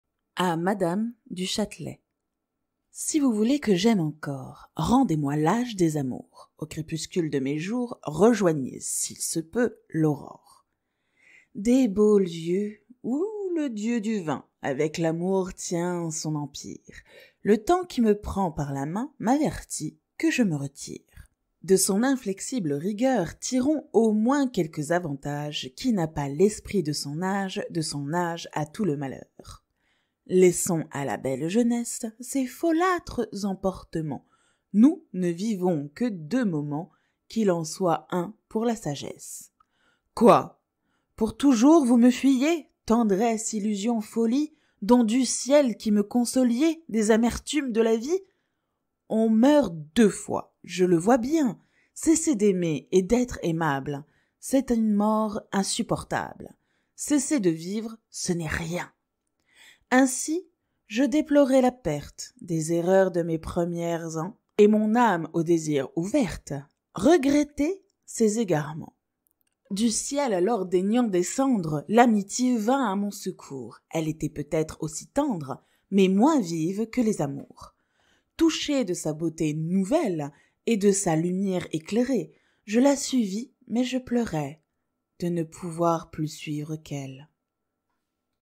Francois Marie Arouet, dit VOLTAIRE - A Mme du Chatelet
"A Mme du Chatelet" est un poème signé Voltaire
Vous y retrouverez ce texte, mais aussi tous les autres poèmes de l'auteur. Mais aussi les écrits de bien d'autres poètes encore...